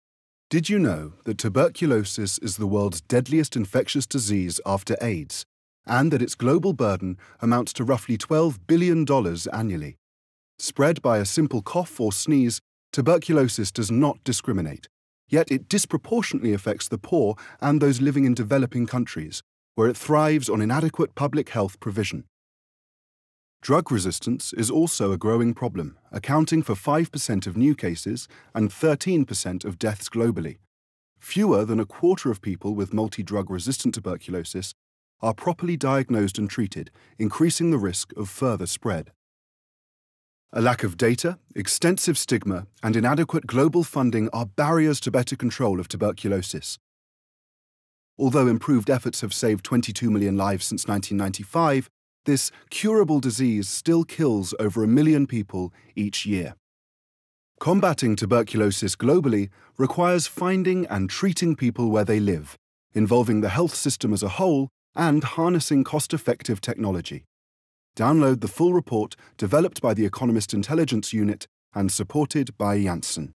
Ancient enemy, modern imperative. A time for greater action against tuberculosis
This Economist Intelligence Unit video, supported by Janssen, investigates the health challenge posed by tuberculosis.